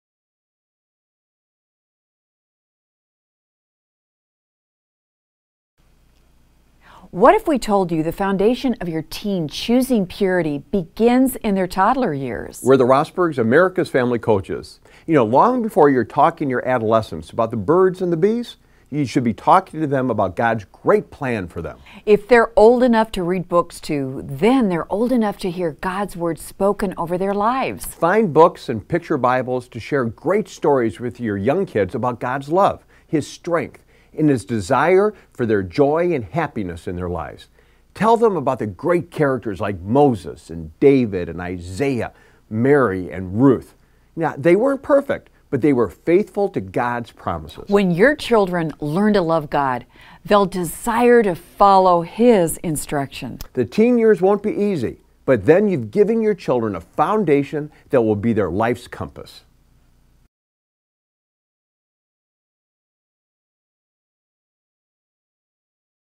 Your Teen's Purity
February 17, 2011 -- When children learn to love God, they will desire to follow His instruction. What a great foundation when battling through the teen years.